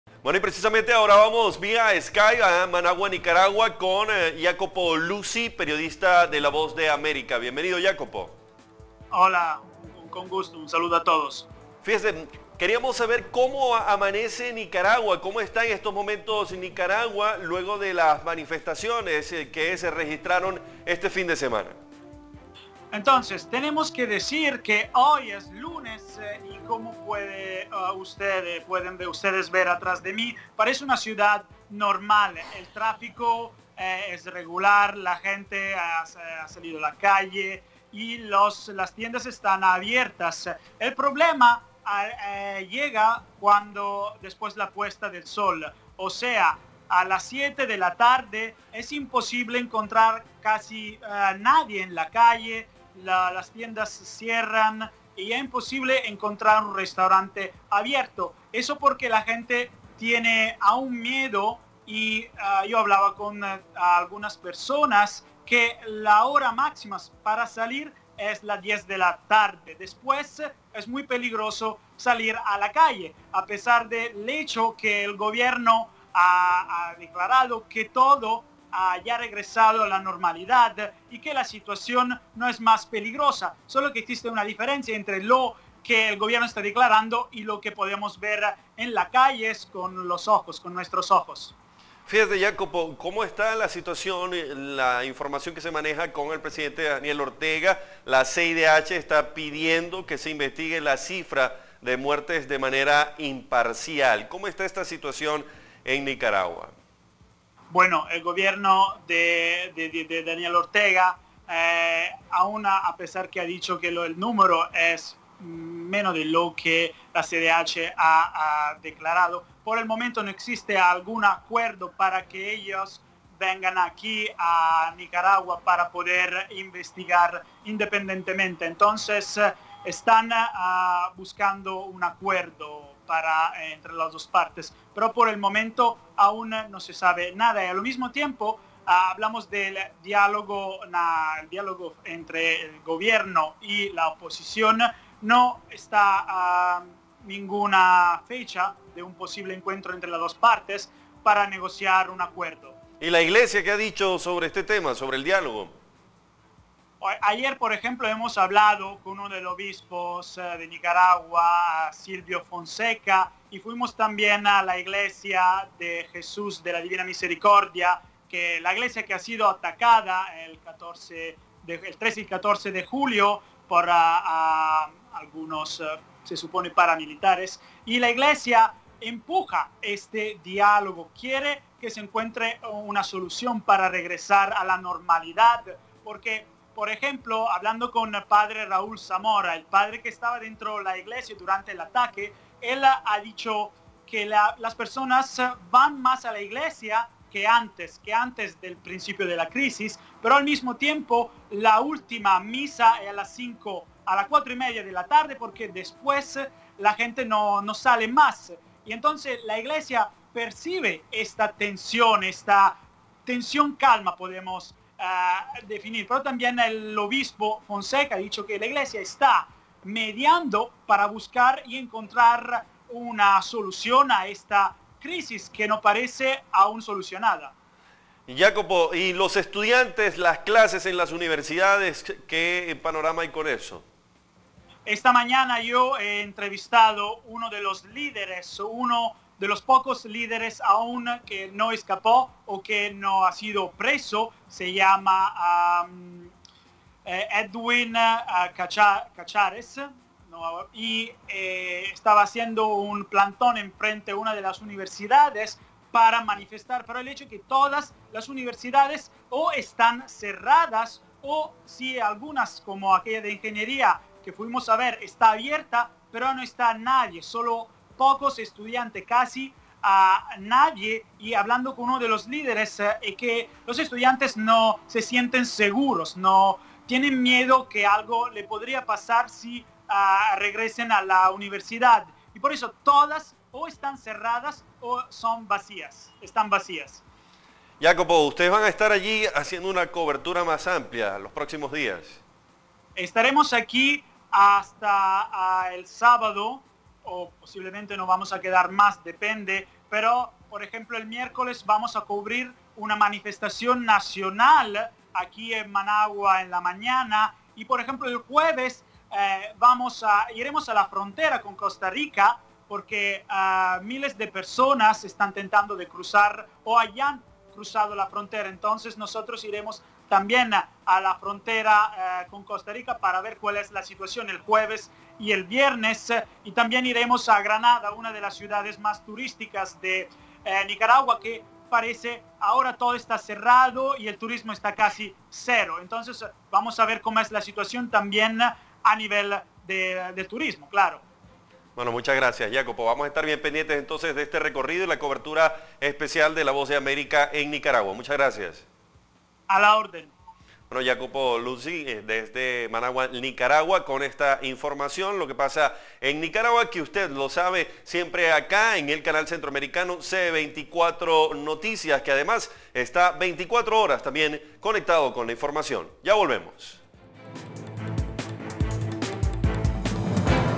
Clima de tensión impone especie de toque de queda en Nicaragua
Contrario a los que se ha difundido en medios de comunicación oficiales, en Nicaragua la calma que se vive actualmente, se ve opacada por el temor de los pobladores a estar en las calles cuando cae la noche.

En un contacto con el periodista de la Voz de América, Iacopo Luzi, el enviado especial revela el testimonio de muchos habitantes que describen como la tensa calma ha impuesto una especie de toque de queda en las calles de Managua y otras ciudades del interior del país, donde la gente prefiere no salir y resguardarse de la acción de paramilitares.